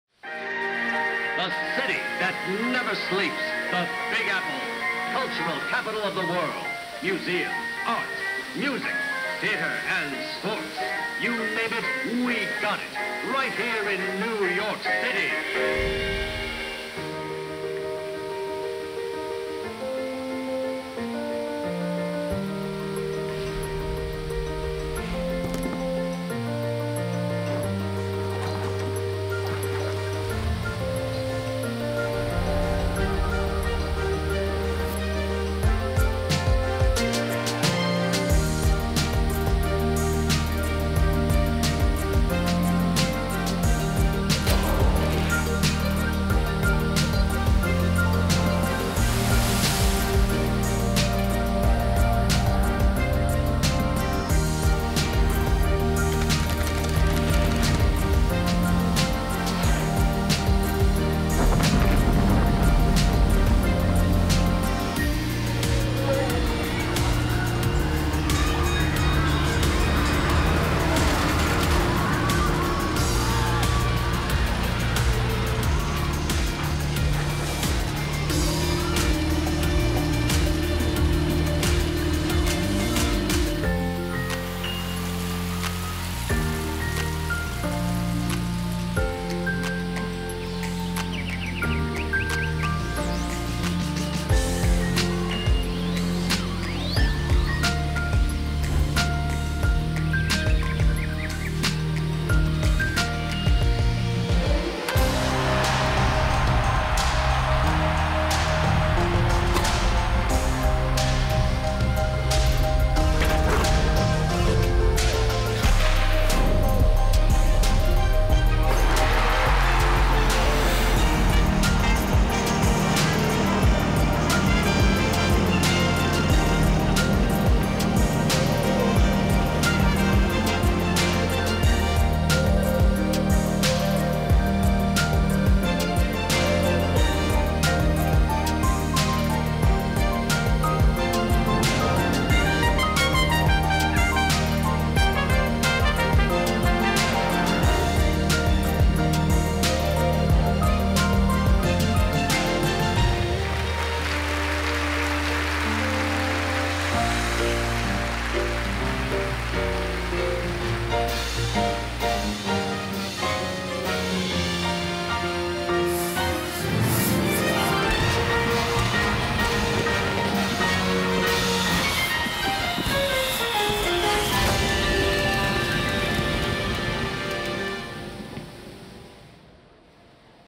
Terminal B Water Show , "Arts and Entertainment"
The Terminal B Water Feature is the only one of its kind in the Americas and the only one of its kind found globally in an airport. 4,000 gallons of recirculated water run through the system and fall from two rings to create a water curtain for Terminal B to project light onto. In addition, 450 nozzles individually open and close to create shapes falling from the water. Our Arts and Entertainment show is a celebration of all there is to do in NYC, the city that never sleeps! Tag us